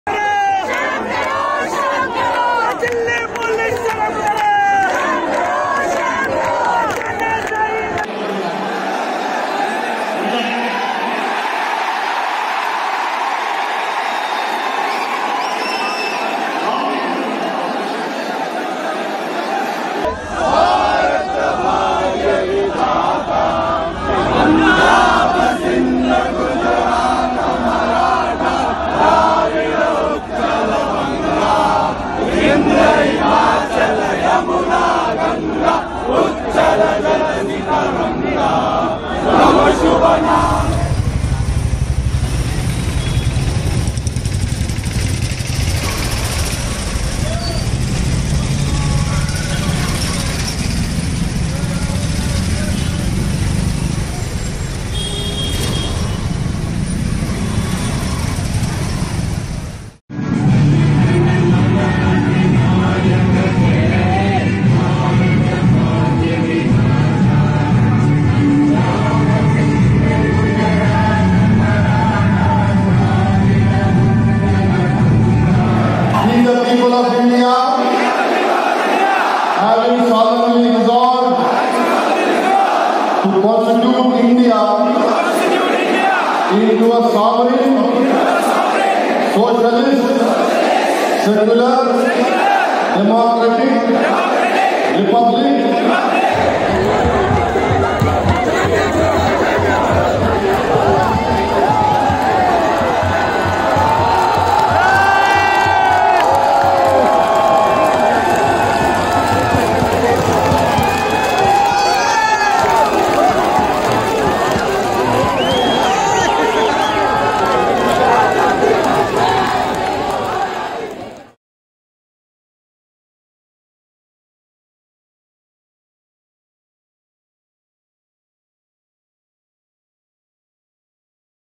5 Unforgettable Moments From Anti-CAA Protests Across India
The protests against NRC and CAA have been raging across the country. A few moments from the protests went viral on social media.